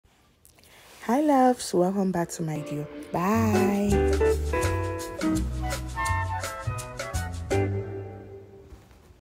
MY TECHNIQUE ON HOW TO PLACE LONG LASTING AND POPPING PIGMENT
Hi loves
Product deets:
Transition shade : Morphe bronzer in a trailblazer
Darker shade: Morphe bronzer in Prodigy
Eyeshadow pallete: Morphe 3502
Eye pigment: luztacosmetics in CHIC

 Eyeshadow brushes : Viccabel

 Lashes : Voanbeauties 10 in one lashes